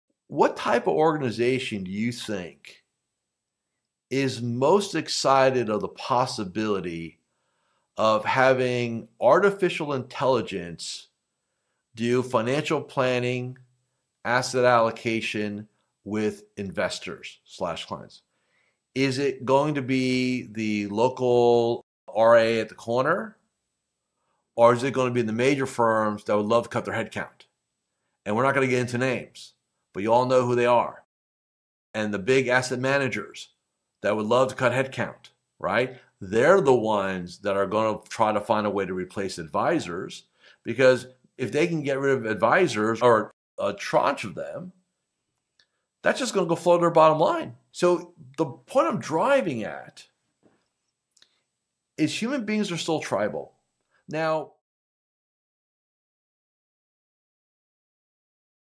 Can Big Firms Cut You Out? AI & The Future of Financial Advice
AI is changing the financial planning game. Surprisingly, it's not big firms seeking savings that benefit most. Instead, local advisors thrive.

Why? Because despite automation, people still value personal connections—something AI can't match.

While AI is great at crunching numbers, it's the empathy of local advisors that clients appreciate.

So, while big firms aim to cut costs, local advisors prioritize relationships. In a data-driven world, personal connections matter most.

📌Check out Magellannetwork.net, where you can find tons of masterclasses forms, tools, templates, and strategies that will help enhance your advisor game.